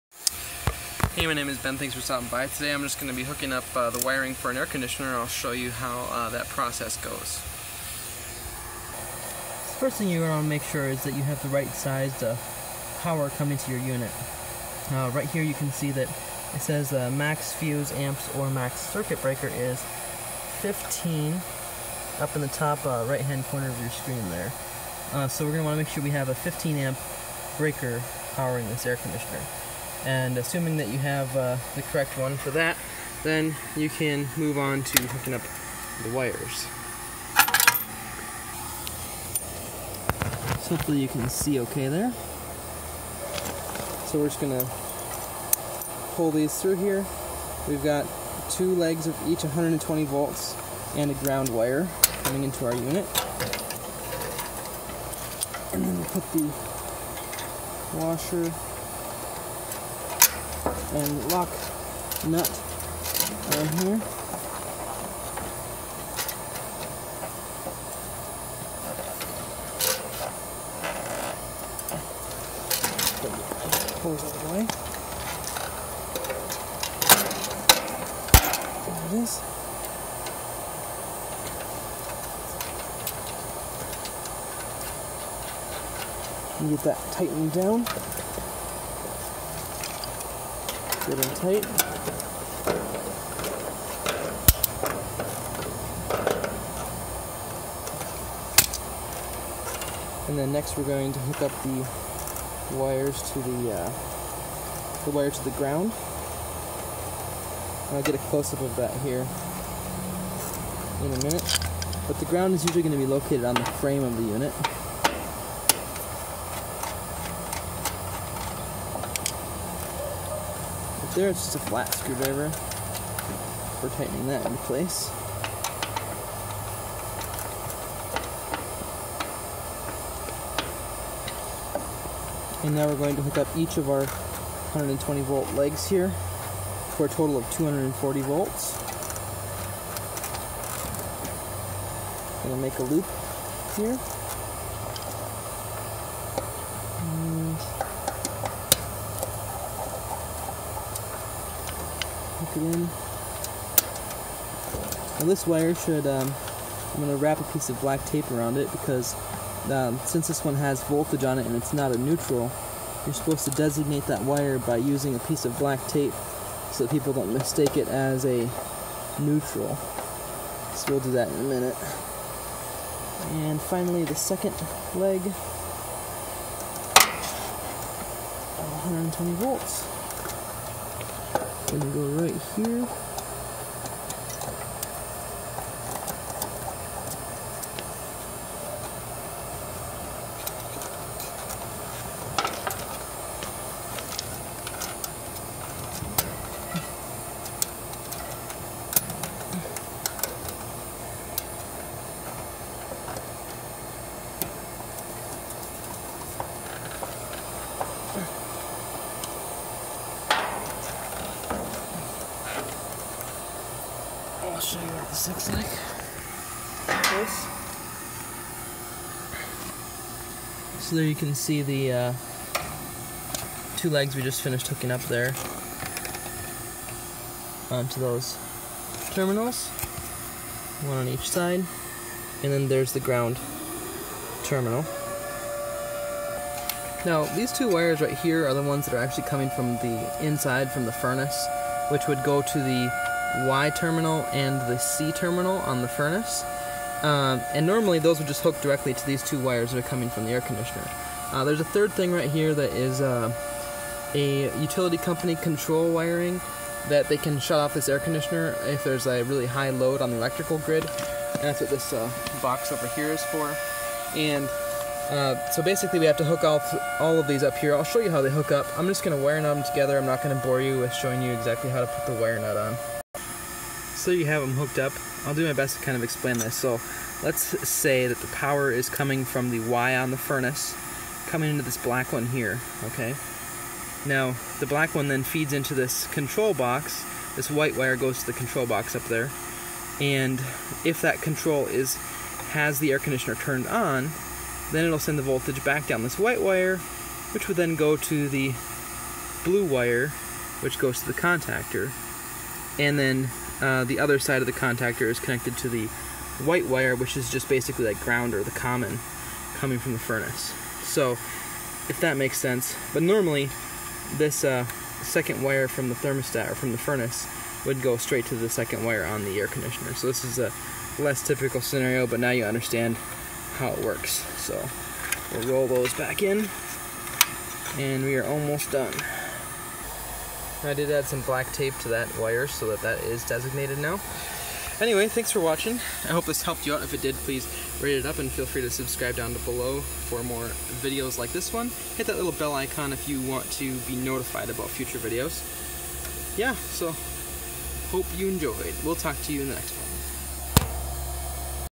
How To Wire an Air Conditioner Condenser Outside Unit - High & Low Voltage AC Wiring
KNIPEX Cobra Pliers 10"
Wiha Insulated Screwdriver Set

Tools We Recommend

Quick video stepping you through how to wire a typical 220v AC condenser unit. Hope it helped you out!

0:00 -  AC Outdoor Unit Wiring Intro
0:09 - Appropriate Amperage Components
0:43 - Pull Power Cable Into AC Unit & Fasten
1:40 - High AC Voltage Wires
3:57 -  Low Voltage AC Control Wires
5:35 - SUBSCRIBE!!!

Blessings,

Ben